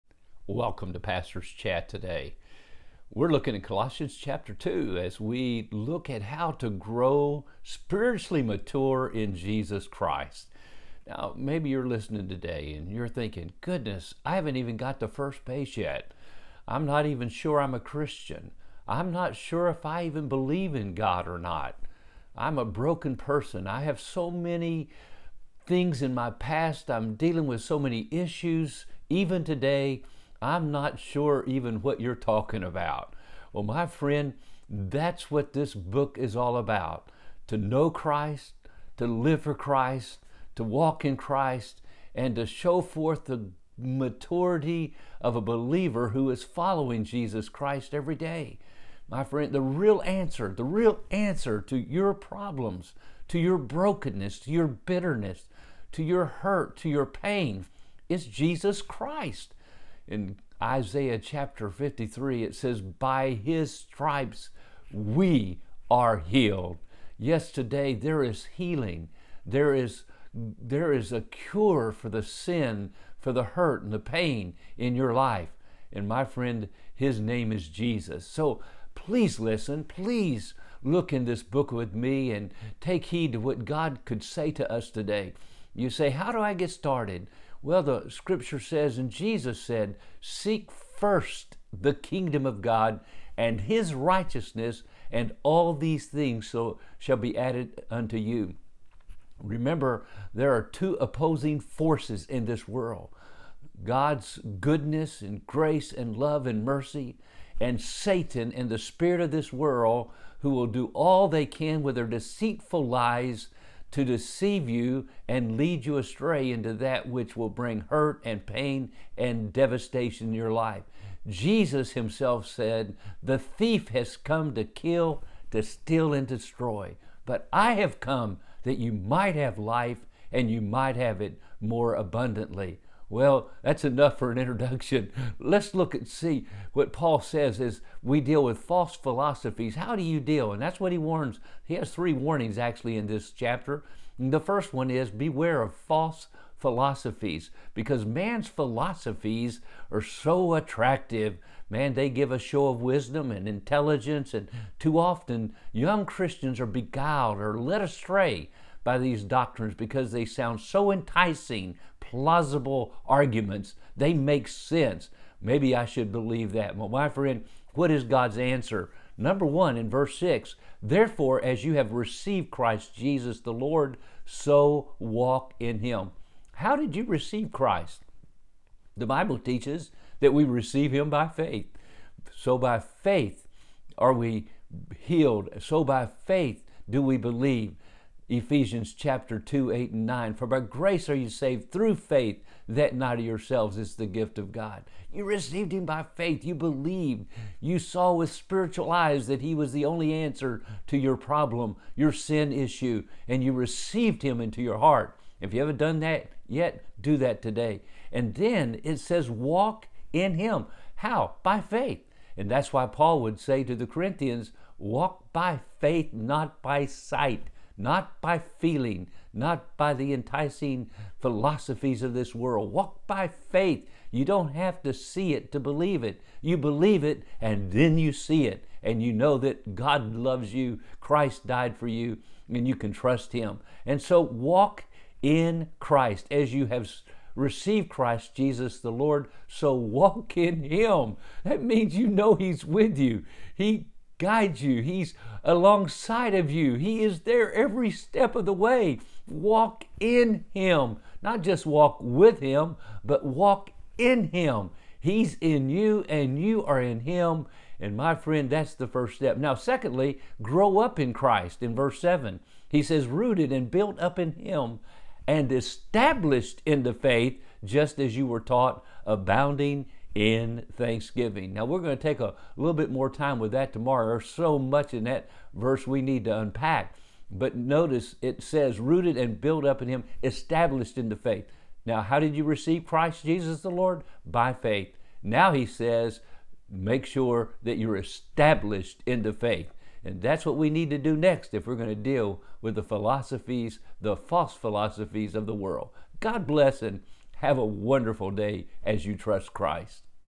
Pastor Mike Chat June 01 2021
Today, Tuesday June 1
The First Step
Colossians 2:5-7
“For though I am absent in the flesh, yet I am with you in spirit, rejoicing to see your good order and the steadfastness of your faith in Christ. As you have therefore received Christ Jesus the Lord, so walk in Him, rooted and built up in Him and established in the faith, as you have been taught, abounding in it with thanksgiving.”

It is so obvious today that we live in a fallen and broken world. On the outside so many of us look just fine, like we don’t have a problem in the world, but on the inside we are carrying tremendous emotional and mental burdens of guilt, bitterness, feelings of rejection, and every morning is a major struggle just to get up and face another day!

My friend, I’m convinced with all my heart that God loves you and He is ready and able to meet the deepest needs and pain of your life!  You ask, “Where do I begin?”  Jesus said, “Seek first the kingdom of God and all these things will be added to you.” (Matthew 6:33) God tells us in the Old Testament, “And you shall seek me and find me, when you search for me with all your heart.” (Jeremiah 29:13)

What is the first step? “As you have therefore received Christ Jesus the Lord….” (vs. 6).
How do you receive the Christ Jesus the Lord?  You must receive Him by faith as you turn from your sin and the way of the world.  “For by grace you have been saved by faith, and not of yourself, it is the gift of God, not of works lest anyone should boast.” (Ephesians 2:8-9)

Now Paul says, that in the same way, we should continue to by faith to walk in Him!  Paul told the Corinthian saints that, “We are to walk by faith not by sight” (2 Corinthians 5:7).  This means that every waking moment we by faith, believe and trust Jesus and His Word, and obey and do His will, instead of living on and going by our feelings!

The false, self-serving philosophies of the world sound so reasonable, so logical and so believable that they can deceive us.  I remind you that the devil “has come to steal, to kill and to destroy” but that Jesus came, “to give us life and to give it to us more abundantly” (John 10:10).

It is a choice we have to make every day!  To “walk in Him and with Him” up the narrow way that leads to life, or to follow the multitudes going down the broad path that leads to destruction.  The first step is to by faith receive Jesus as your Lord and Savior.  The second step is to also by faith begin to “walk in Him” every day!

God bless!